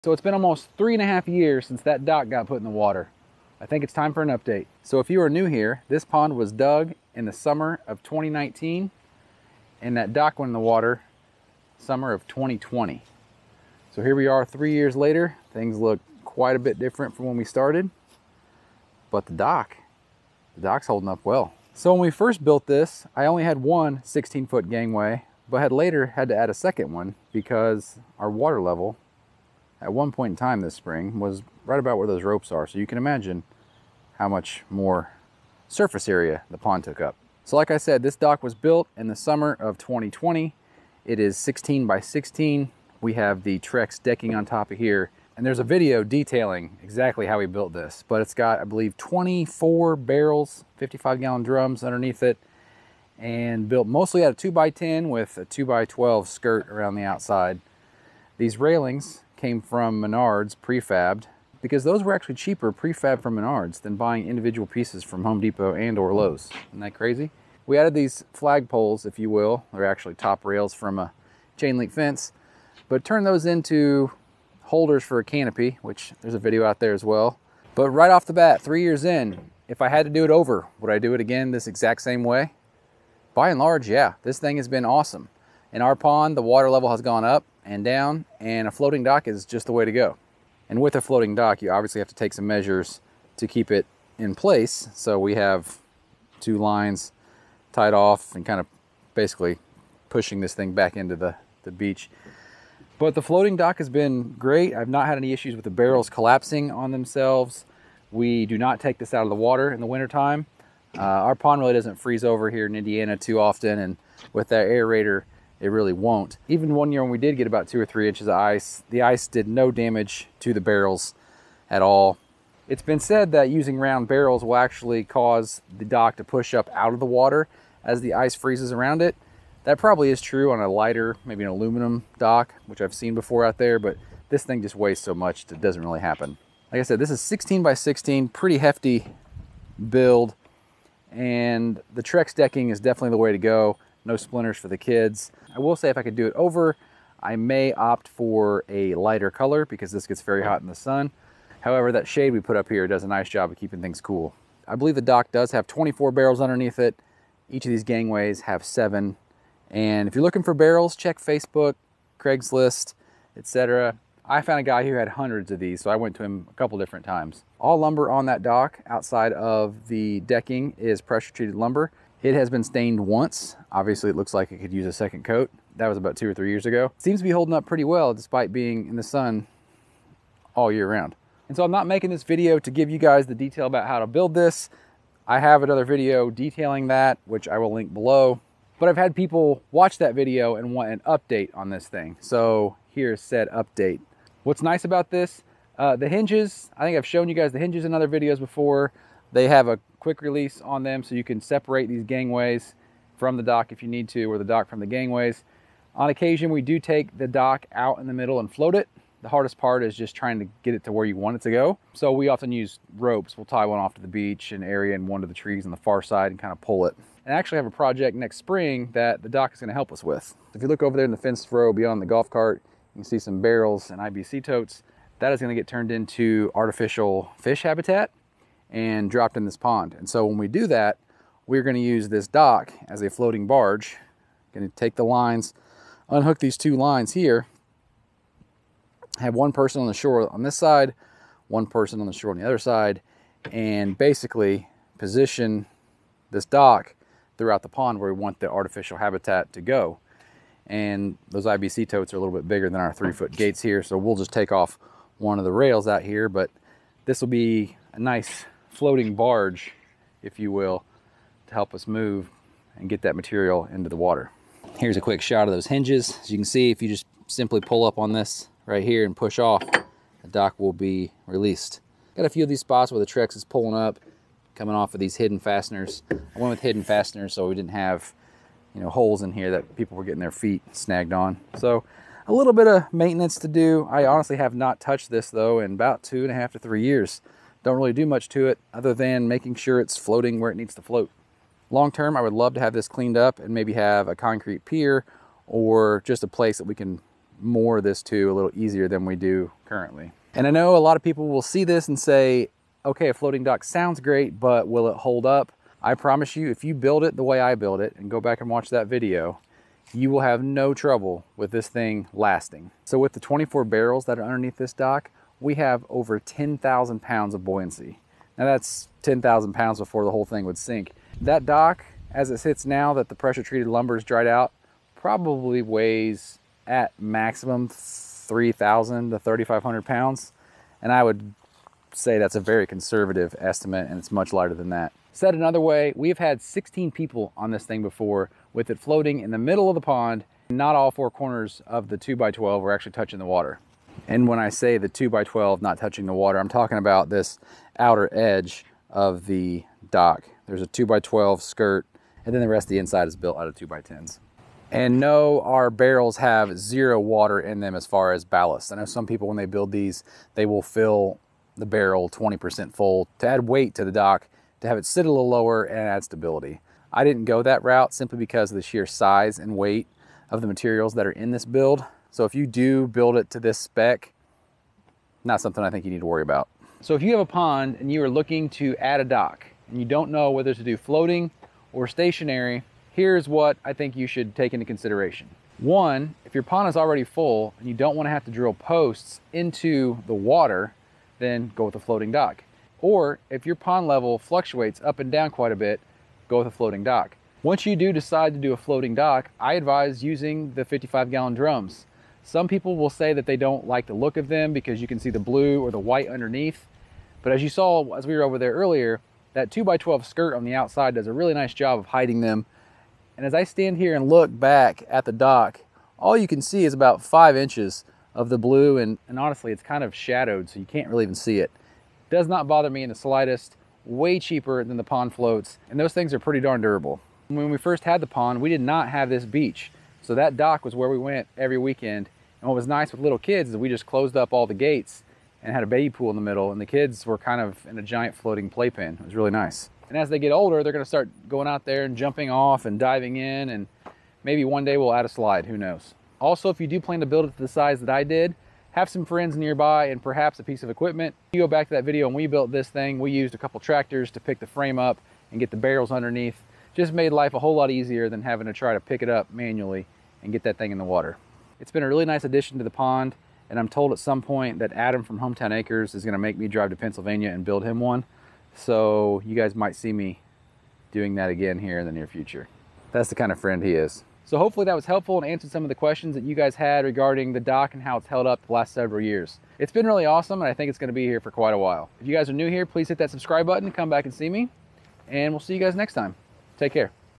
DIY Floating Dock (with plastic barrels) - Three Year Review
The floating dock is 3 years old.  I wanted to give everyone an update as to how it has held up.  It was an awesome addition to our pond and we get a lot of use out of it.  Building it with plastic barrels (55 gallon drums) is the way to go!  A number of build videos around the dock can be found below.

This storefront provides links to many of the products we use around the farm.